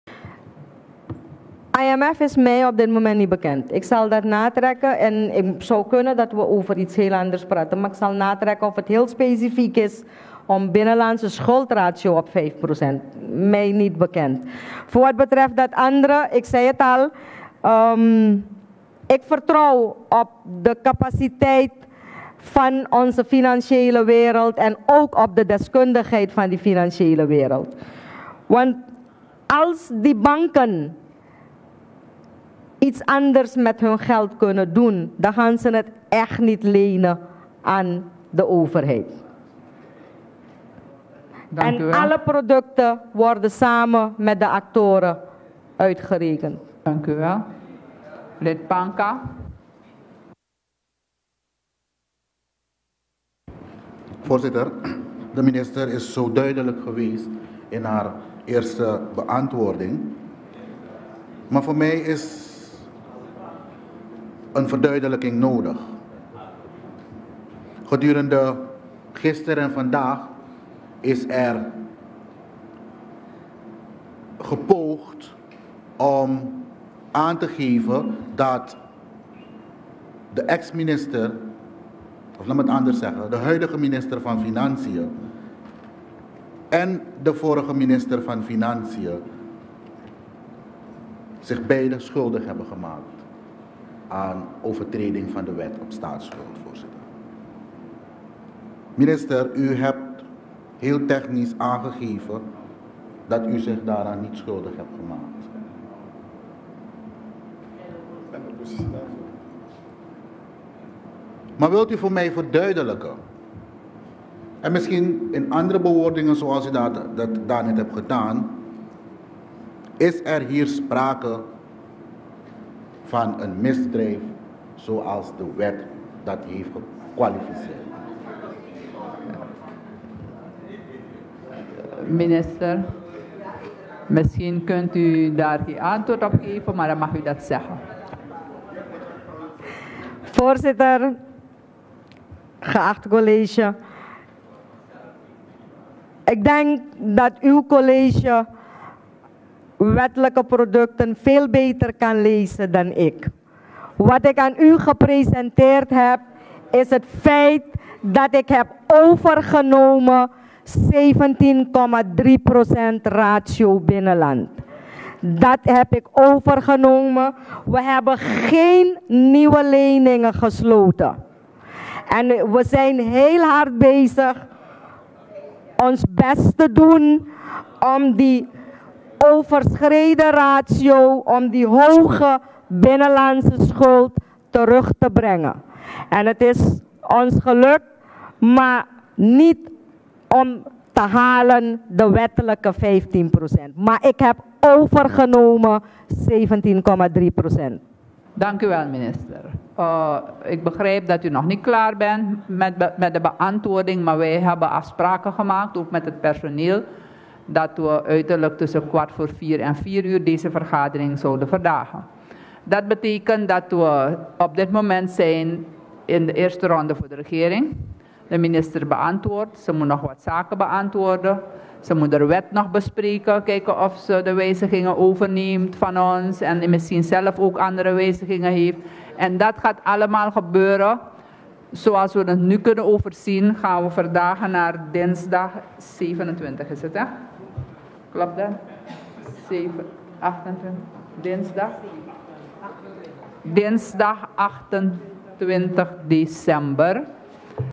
03. Reactie Minister Boedhoe (Deel 4)
Minister van Financiën Wonnie Boedhoe geeft, na kort voorwoord van Vicepresident Robert Ameerali, antwoord op vragen van de leden van het parlement. Het vierde deel.